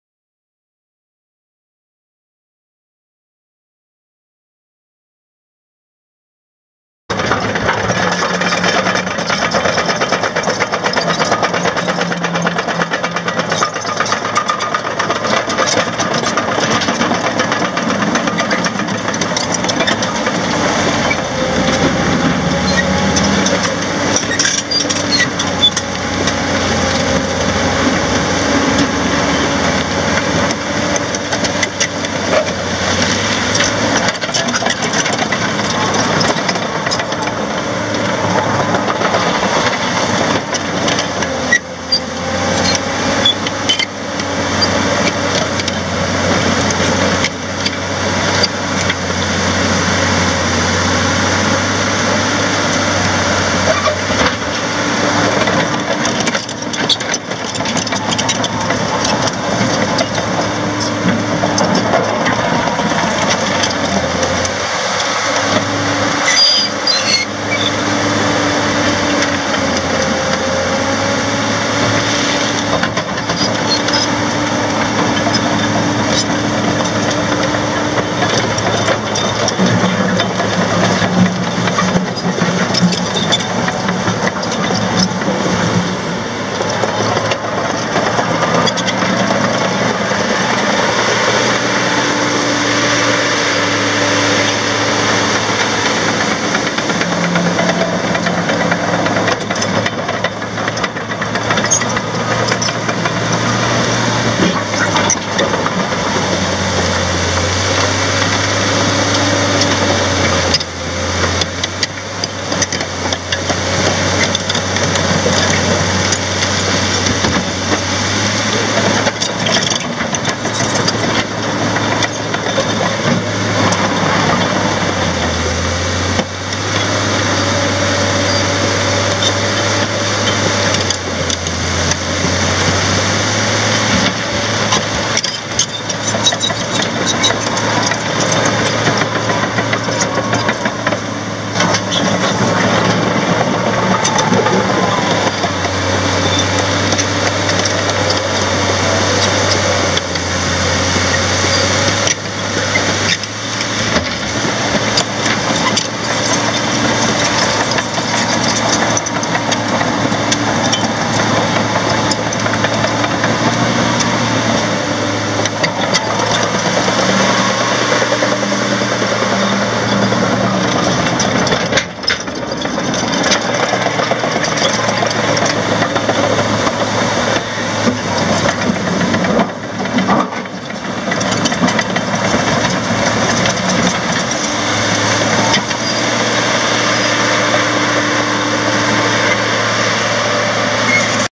2005 Caterpillar 953C track loader for sale | sold at auction February 5, 2015
SOLD! Manhattan Trenching Retirement Auction

2005 Caterpillar 953C track loader
4,531 hours on meter
Caterpillar 3126 six cylinder diesel engine
Hydrostatic transmission
Enclosed cab
AC and heat
AM/FM/weatherband
Joystick controls
94"W bucket
Eight bolt-on teeth
Bolt-on cutting segments
Six bottom rollers
19.5"W tracks
33,389 lbs. base operating weight
Service records available
Serial CAT0953CABBX02096